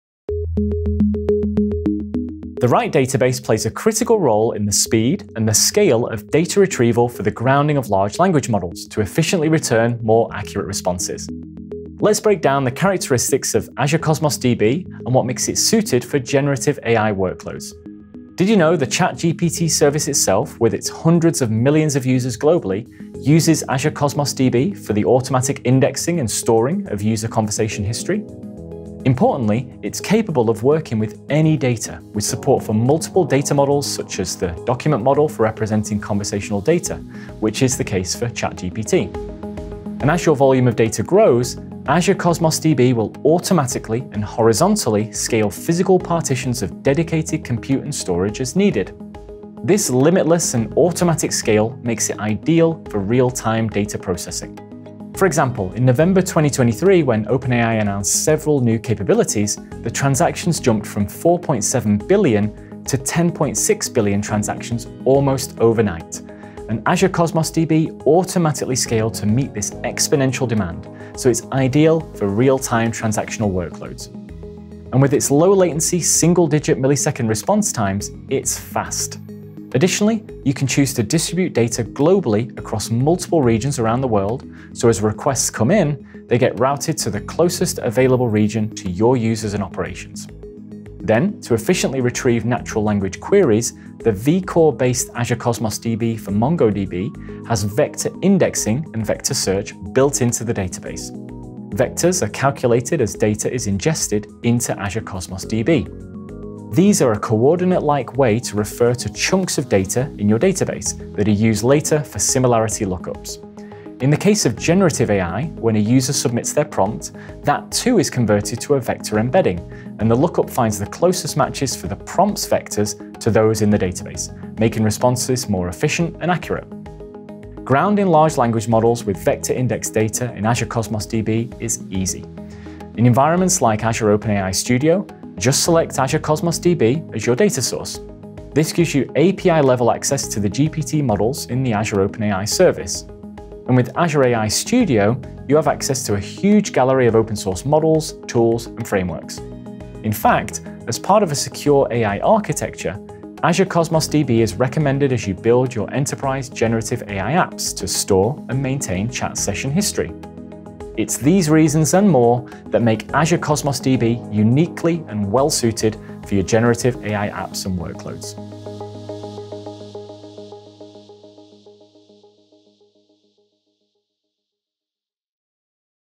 Generative AI with Azure Cosmos DB
Leverage Azure Cosmos DB for generative AI workloads for automatic scalability, low latency, and global distribution to handle massive data volumes and real-time processing. With support for versatile data models and built-in vector indexing, it efficiently retrieves natural language queries, making it ideal for grounding large language models. Seamlessly integrate with Azure OpenAI Studio for API-level access to GPT models and access a comprehensive gallery of open-source tools and frameworks in Azure AI Studio to enhance your AI applications.

► QUICK LINKS:
00:00 - Azure Cosmos DB for generative AI workloads
00:18 - Versatile Data Models
00:39 - Scalability and performance
01:19 - Global distribution
01:31 - Vector indexing and search
02:07 - Grounding LLMs
02:30 - Wrap up

► Link References

This is Part 1 of 3 in our series on leveraging generative AI.